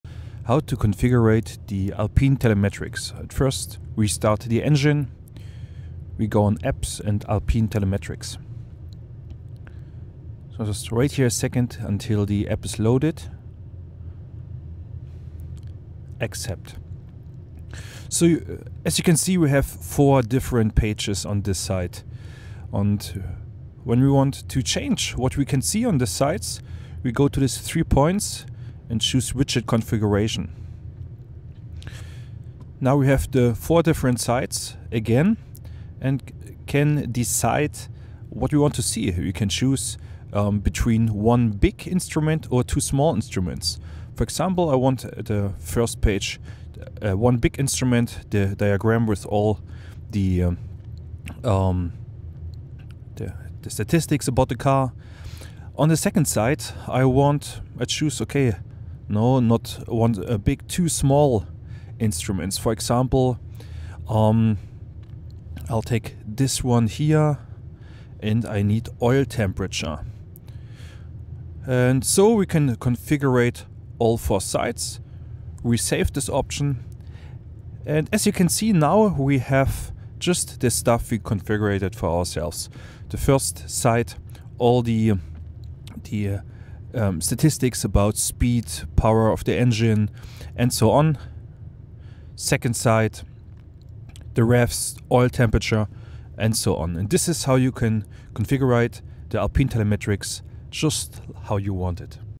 Alpine 2022 Telemetrics Configuration - Tutorial
Numerous Questions reached us about the configuration of the alpine telemetrics with the release of the facelift version 2022. At the beginning, ourselves struggled also with the new app, so we decided to give you a small tutorial in how to suite the telemetrics app to your needs.